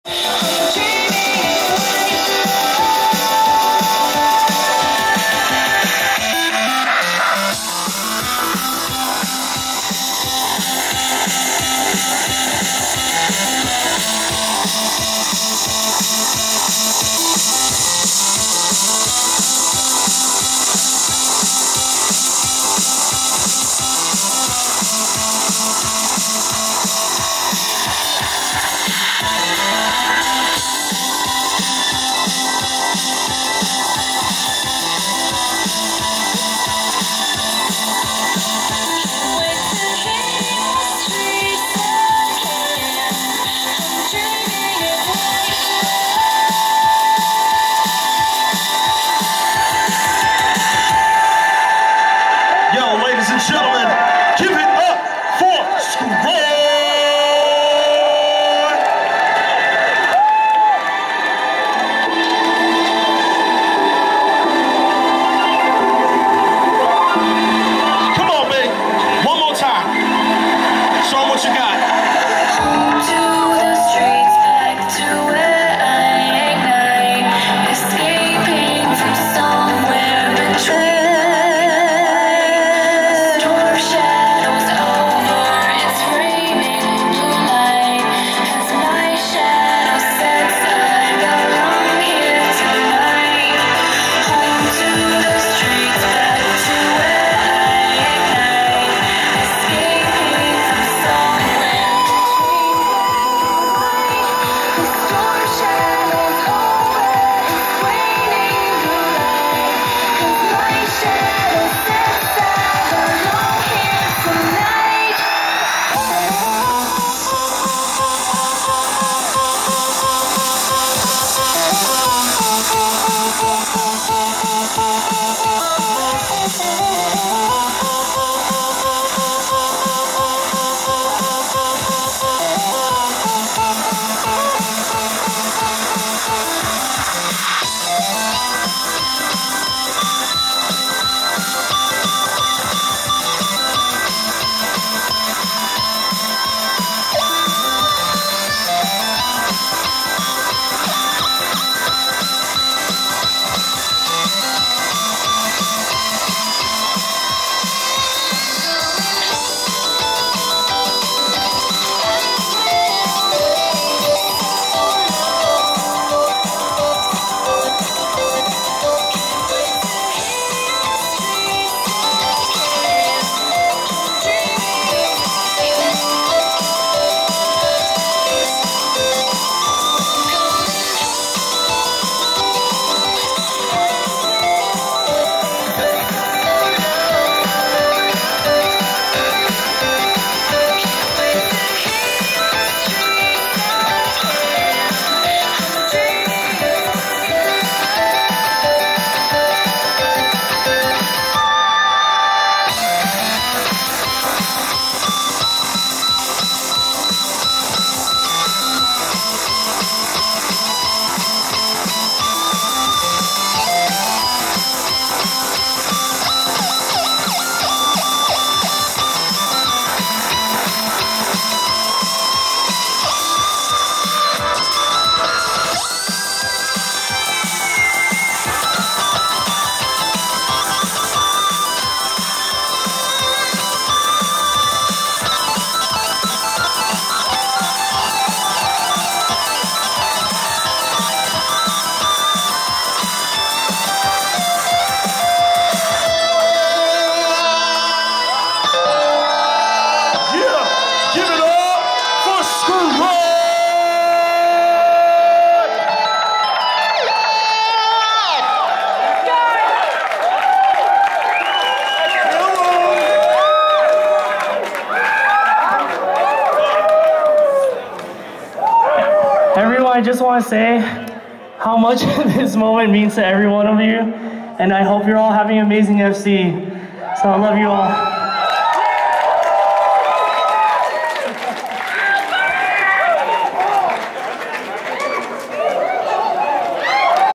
Further Confusion 2014 Dance, Skroy and Rawkz0rz!!
Further Confusion 2014, Saturday into Sunday (January 18-19) dance, Skroy (on Keytar) and Rawkz0rz!! DJing.  Filmed with a Sony RX100 at 1080 60i.